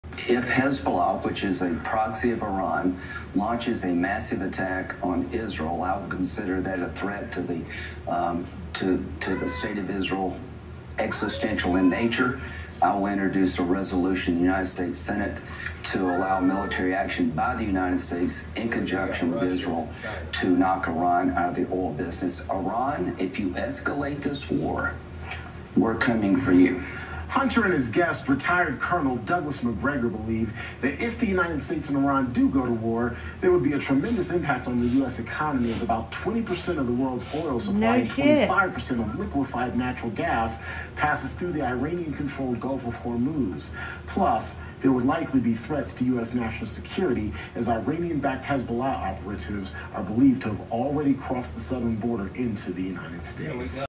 Lindsey Graham & Colonel Douglas Macgregor talking key points about WWIII between U.S. & Iran
Gives you a dialogue & perspective of what started to happen under Biden Administration and what’s happening now & what could happen in the future.